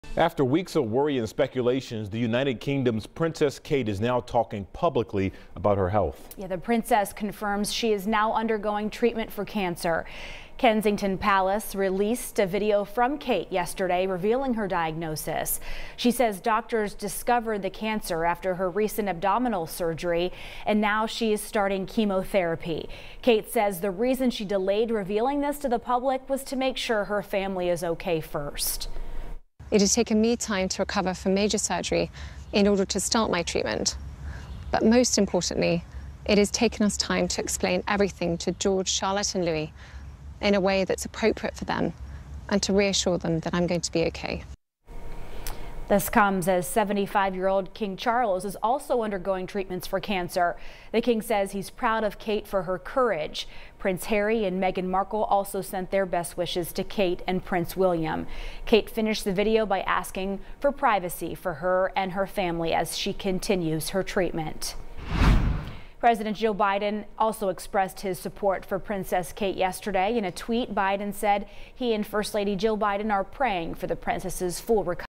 Princess Kate speaks out on her cancer diagnosis, chemotherapy
Kate says the reason she delayed revealing this to the public was to make sure her family was okay.